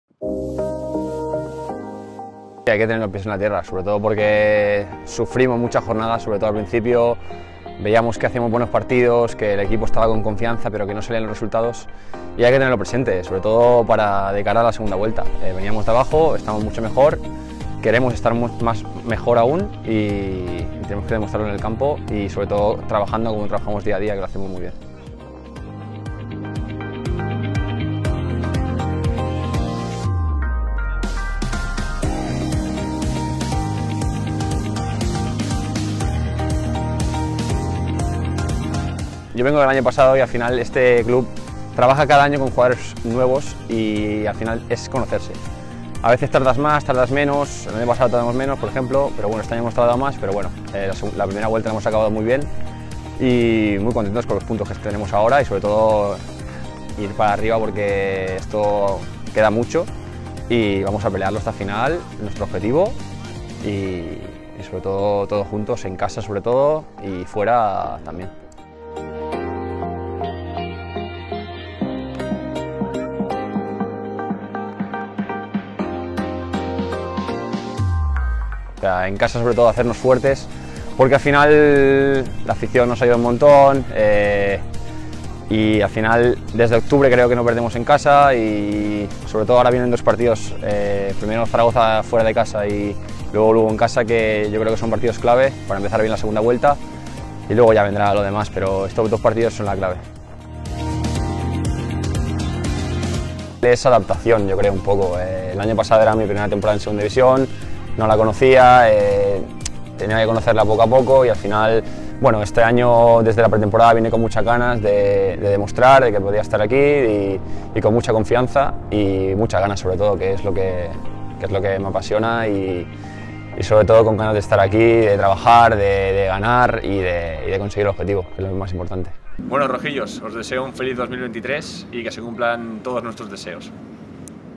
"El equipo ha vuelto con muchas ganas"
Oriol Rey repasa el momento del equipo, que ya se ejercita tras el parón navideño.